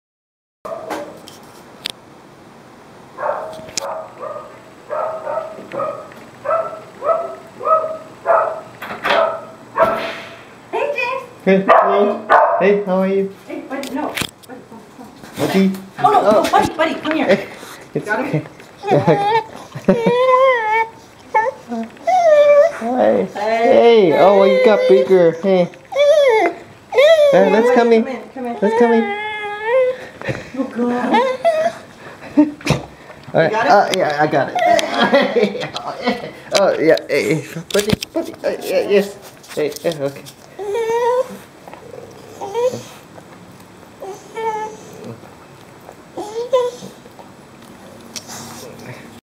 Buddy Greeting Me
My friend's dog, Buddy, was very happy to see me when I visited the other day...! Reminds me of my own dog, Rocky, who passed away recently but looked almost exactly like Buddy when he was a puppy.